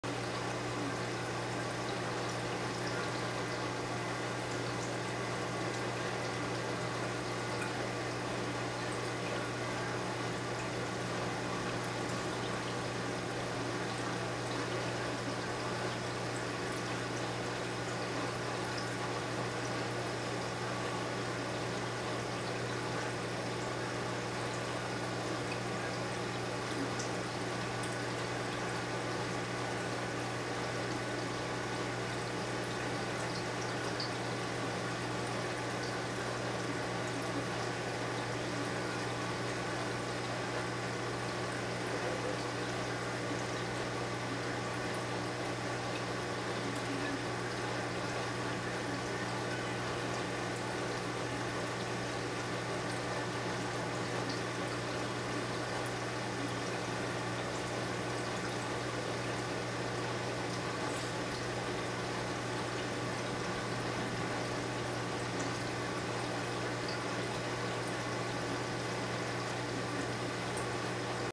Playful pink streaked wrasse
He gets very active as the light starts to dim.  Unfortunately he's still a little shy with the camera but this gives you an idea.  If the camera wasn't there, he'd come right up to me at the glass and stare and play...he's adorable.